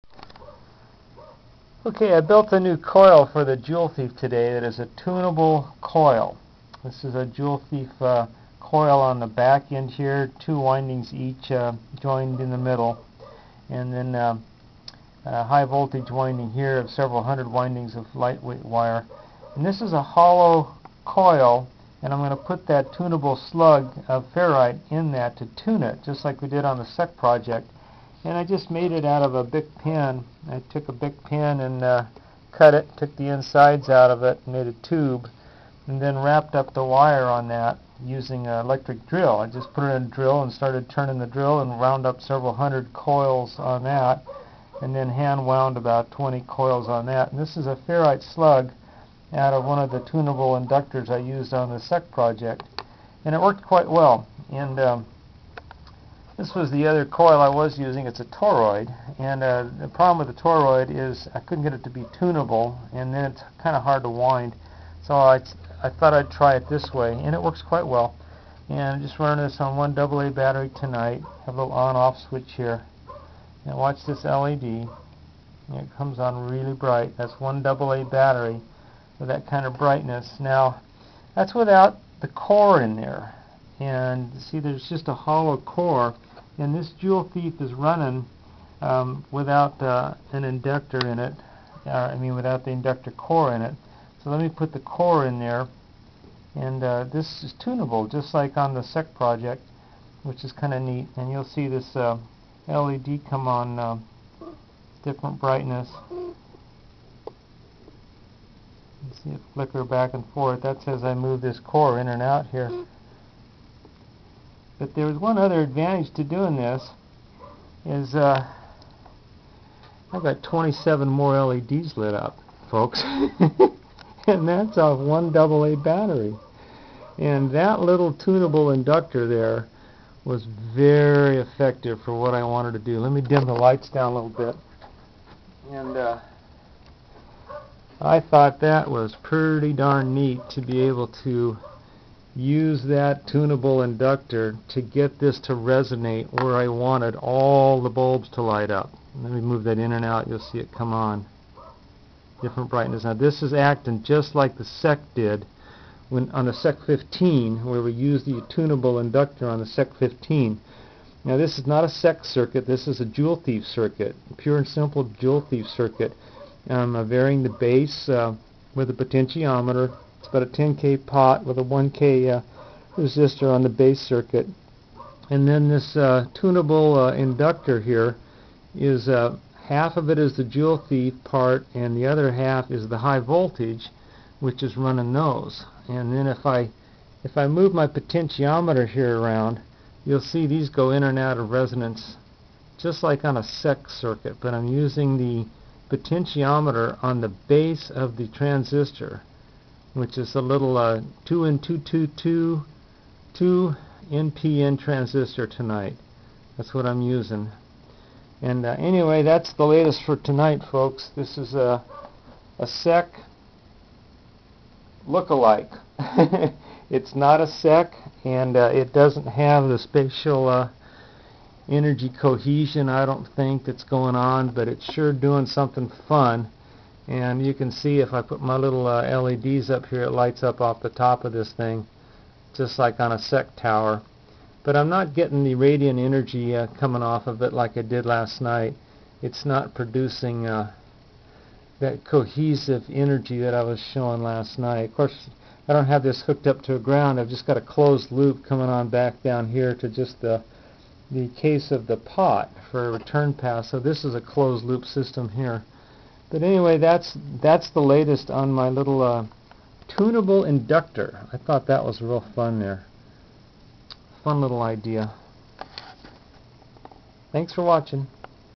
Joule Thief-- with tunable coil
This is a standard Joule Thief circuit with a tunable inductor coil.  The simple one transistor circuit is powered off one 1.5 volt AA battery and is driving multiple LEDs in two locations.   The coil is hollow and a ferrite slug is used to adjust it's three windings into resonance.  A potentiometer at the transistor base is also used to fine tune the circuit.  The 27 LEDs are on a Dr. Stiffler SEC tower and are-- IN SERIES!  The one LED on the board is in the standard location off the collector.  All of the LEDs can be made to light at the same time.